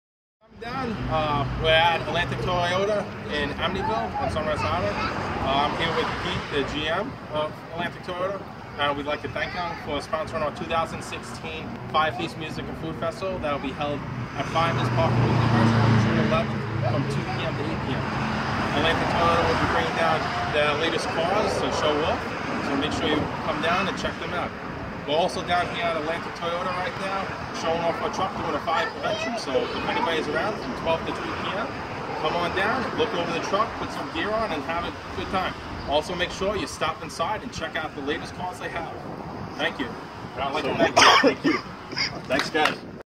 Lindenhurst Firefighters
We are doing a fire prevention clinic today until 3pm.  Come on down!  Lindenhurst Firefighters are here.